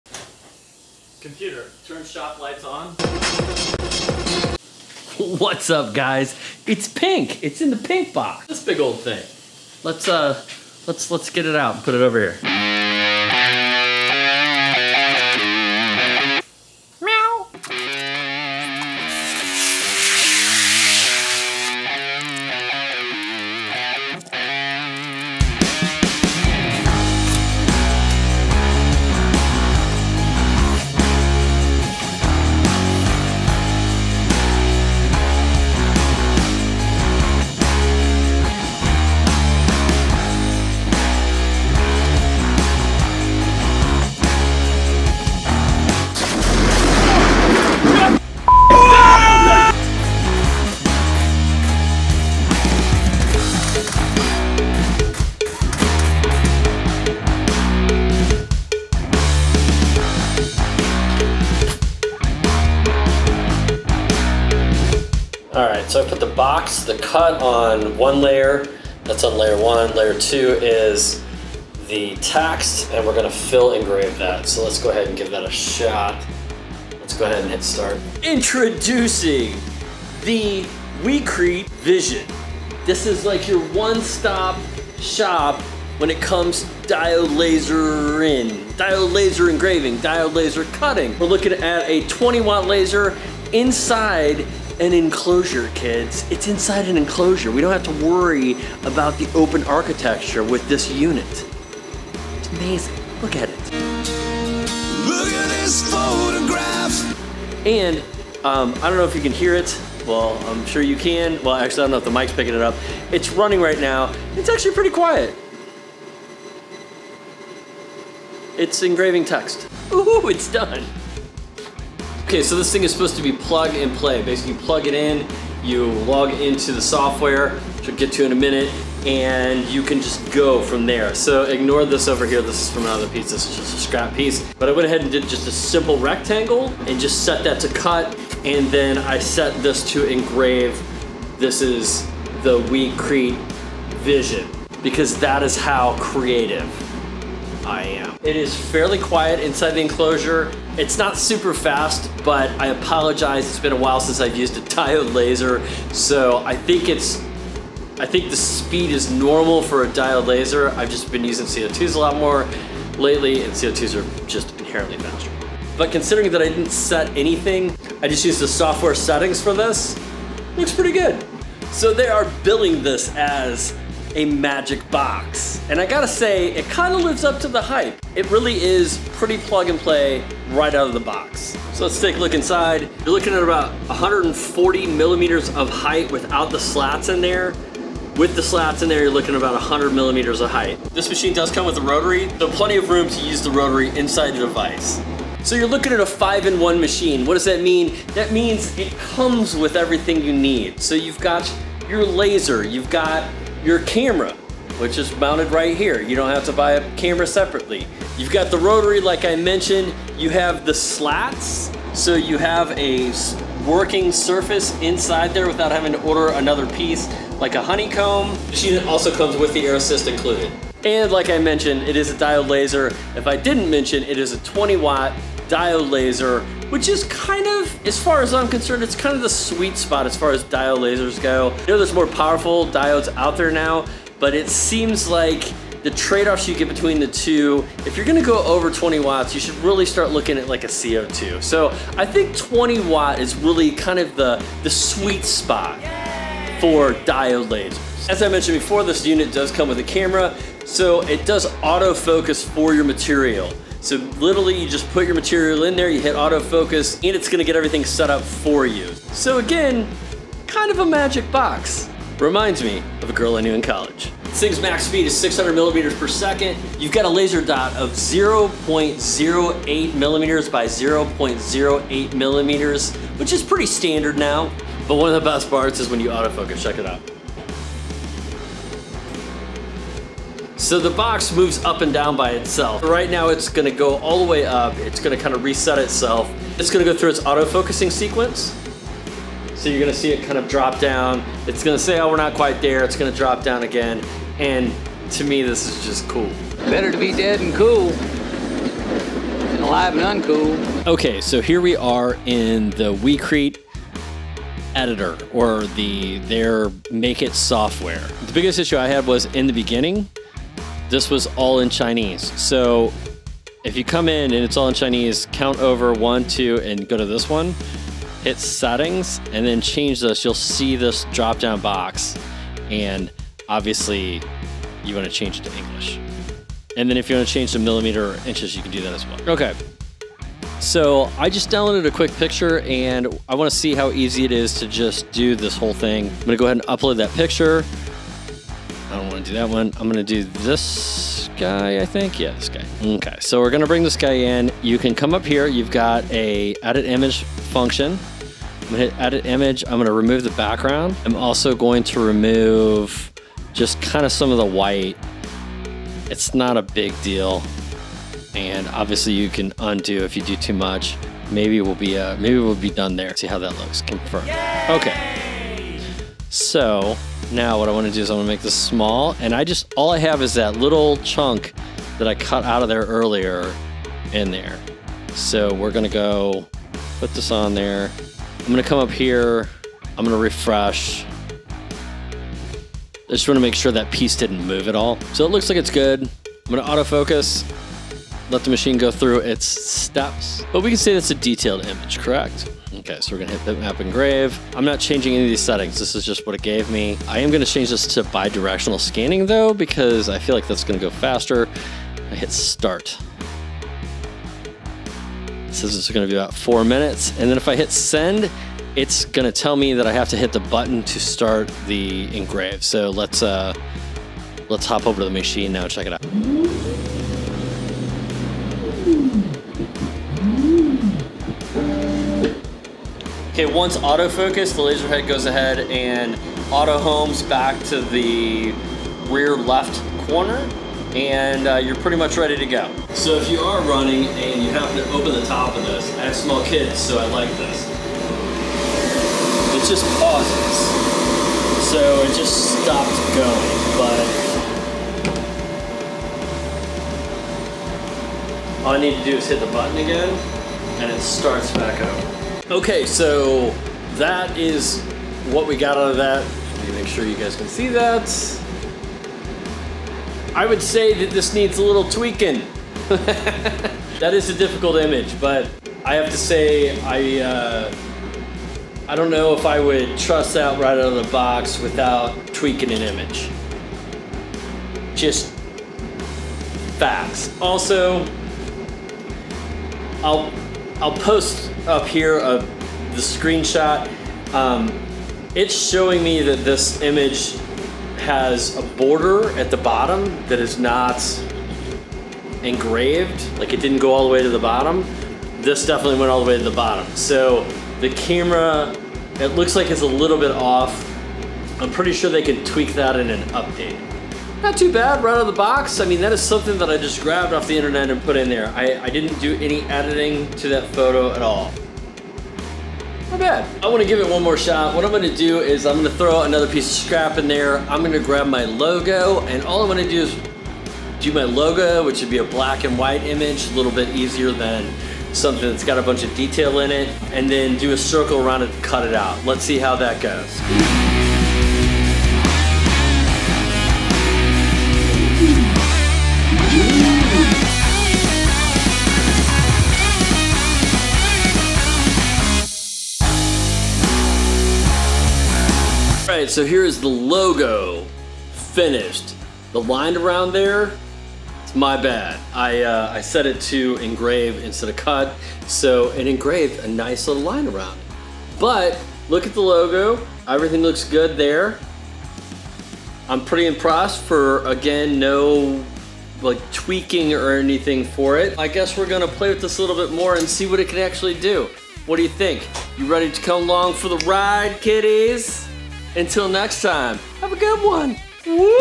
Unboxing the WeCreat Vision All in One 20W Diode Laser
ME STUFF!

Build Dad Build
C/o Nick Vespa